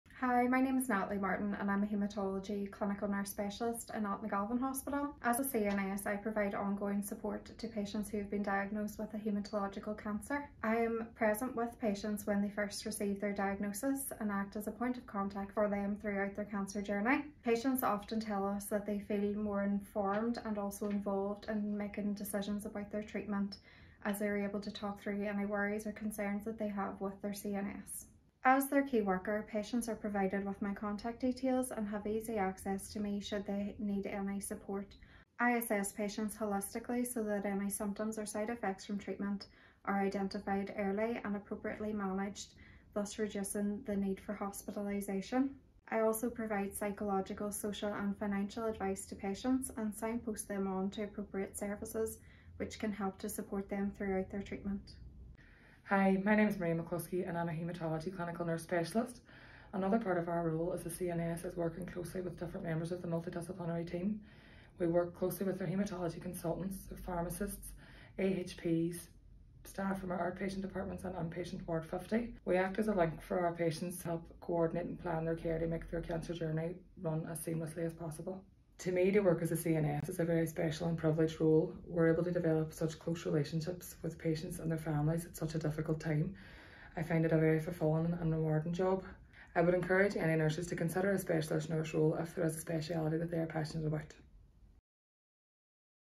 Cancer Clinical Nurse Specialist Day 2022
15 March 2022 marks Cancer Clinical Nurse Specialist (CNS) day.  We in the Western Trust want to celebrate our cancer clinical nurse specialists, raise awareness of the role and recognise the contribution that they make to patients and families as well as to the wider multidisciplinary team.

It is widely recognised that having access to a CNS improves the quality and experience of care for our patients. The role of the cancer CNS is to provide information and support for patients and families regarding their diagnosis and treatment. The CNS acts as a keyworker, assesses the holistic needs of the patient and makes appropriate referrals to other services to ensure that the care provided is tailored to their specific needs.

In the Western Trust, we have 22 cancer CNSs across lung, breast, colorectal, haematology, gynae, urology, upper gastrointestinal, skin and head and neck tumour sites as well as a radiotherapy CNS. Some of our Clinical Nurse Specialists have been kindly funded by Macmillan Cancer Support.